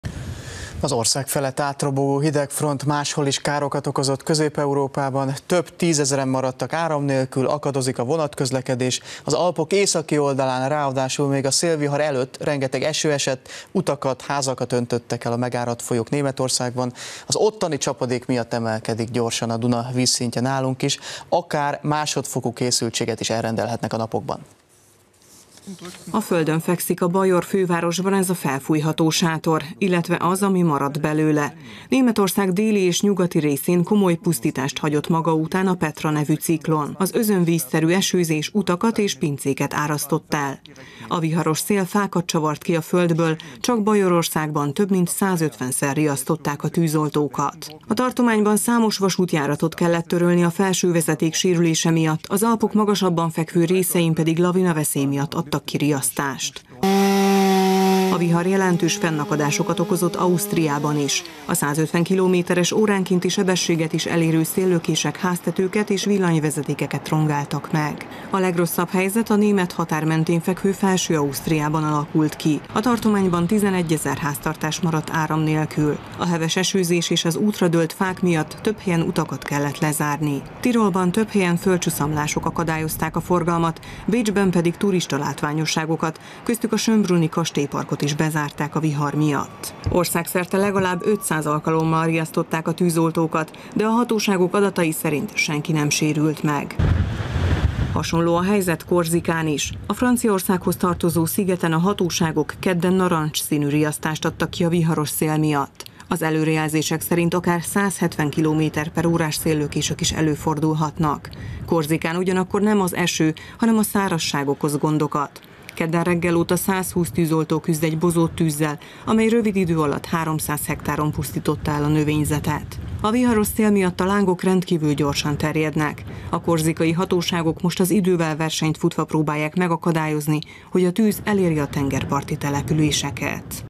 Tízezrek maradtak áram nélkül a viharos idő miatt
A Magyarország felett átrobogó hidegfront máshol is gondot és károkat okoz. Több tízezren maradtak áram nélkül, akadozik a közlekedés Németországban és Ausztriában is. Az Alpok északi oldalán ráadásul még a szélvihar előtt rengeteg eső esett. Utakat, házakat öntöttek el a megáradt folyók. Az ottani csapadék miatt emelkedik gyorsan a Duna vízszintje Magyarországon is, akár másodfokú készültséget is elrendelhetnek a napokban a felső szakaszon.